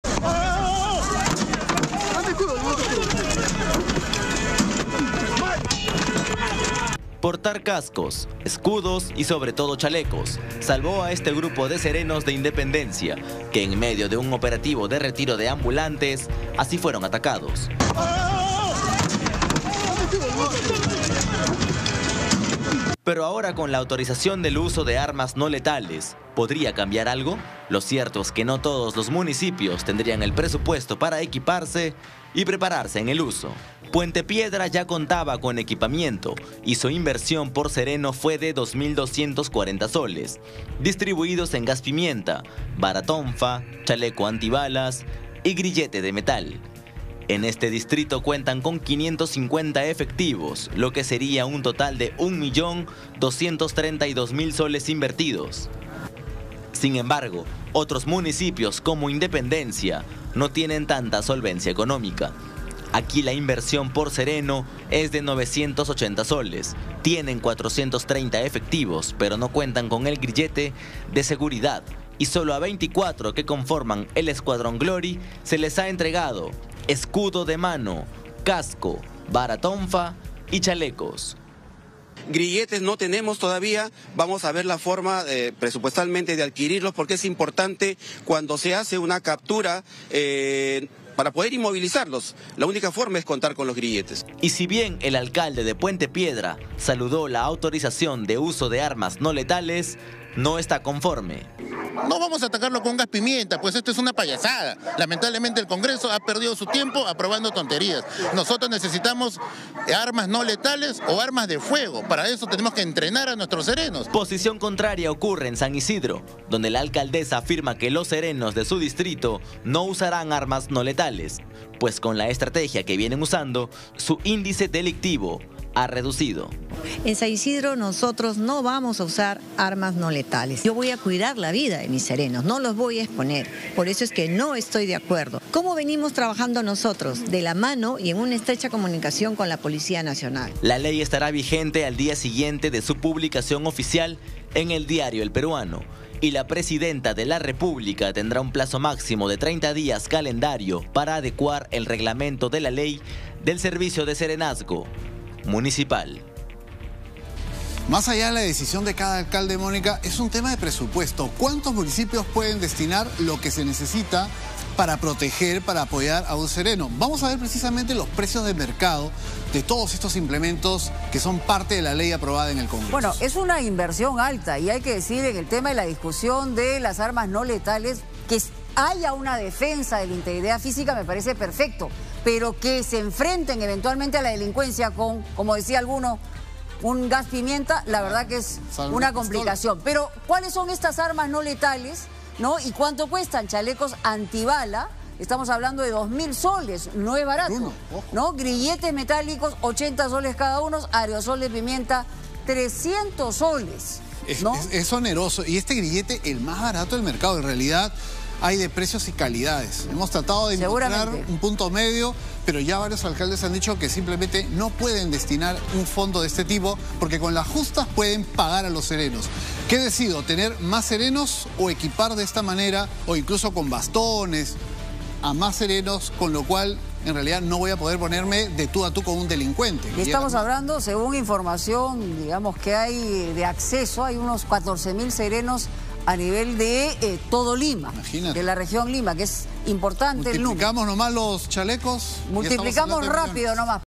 Tras aprobación de uso de armas no letales, ¿cuánto costaría equipar a un sereno?
No todos los municipios contarían con los recursos para equipar a sus agentes.

LatinaNoticias. Encuentra las noticias verificadas y más rigurosas, más información, entretenimiento y variedades